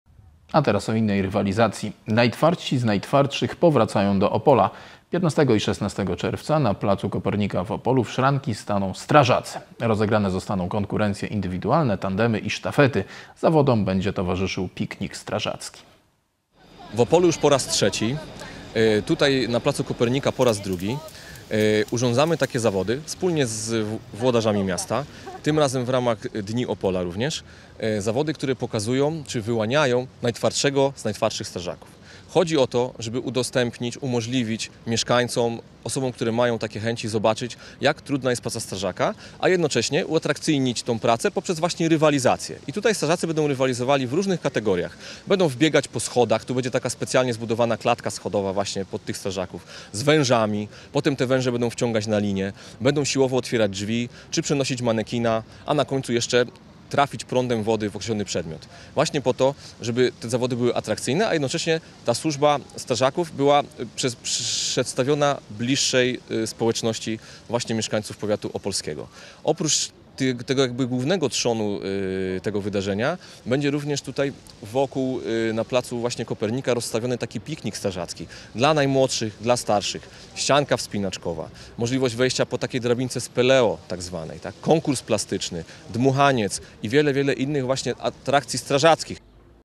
Zawody i piknik z atrakcjami. 15 i 16 czerwca turniej strażacki
Najtwardsi z najtwardszych powracają do Opola. 15 i 16 czerwca na placu Kopernika w Opolu w szranki staną strażacy. Rozegrane zostaną konkurencje indywidualne, tandemy i sztafety. Zawodom będzie towarzyszył piknik strażacki.